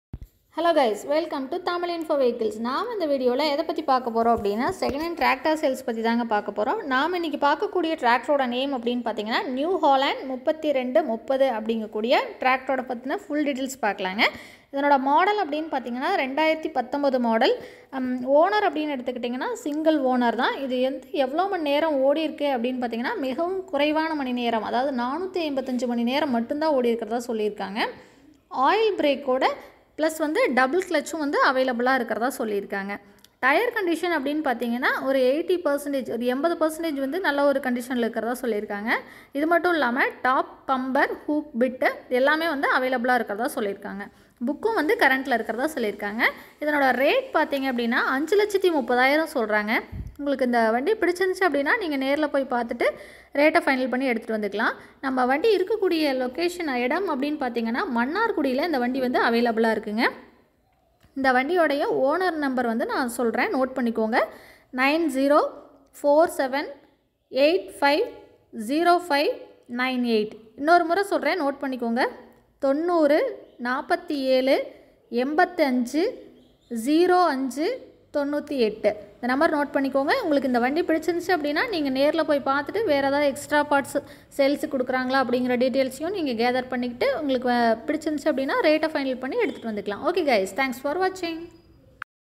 NX 3230 NEW HOLLAND Plus Tractor For Sale 2019 Model
Place : Mannaarkudi
Amount : 530000